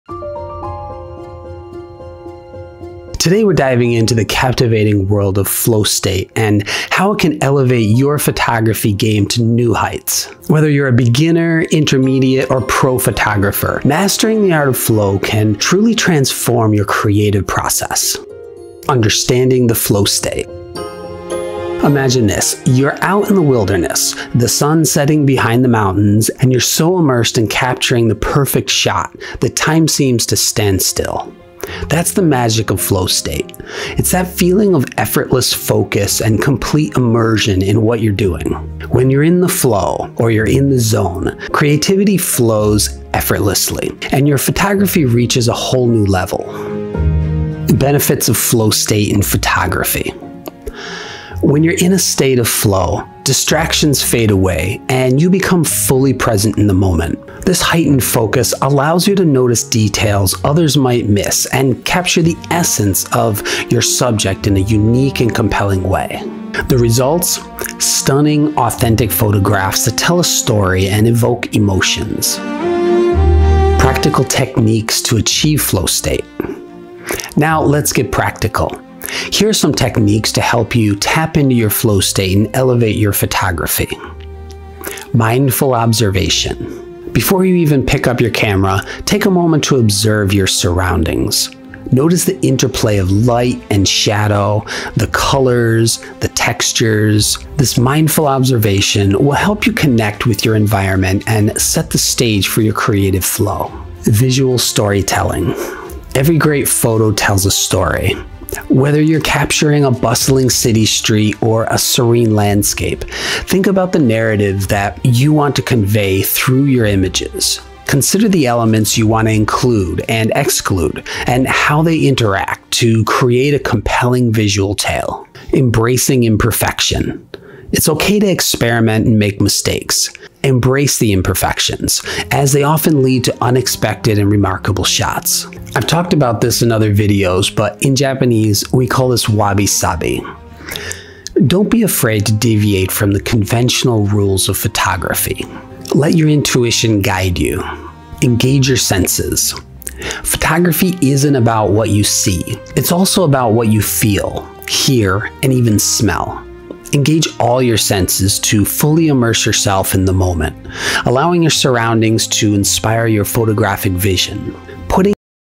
Unlocking Your Creative Flow: Photography Techniques to Capture Perfect Moments with Ease
Today, we're diving into the captivating world of flow state and how it can elevate your photography game to new heights. Whether you're a beginner or an intermediate photographer, mastering the art of flow can truly transform your creative process...

◦ Visit Shizen Style for learning resources like E-books, my blog, newsletter, shopping etc.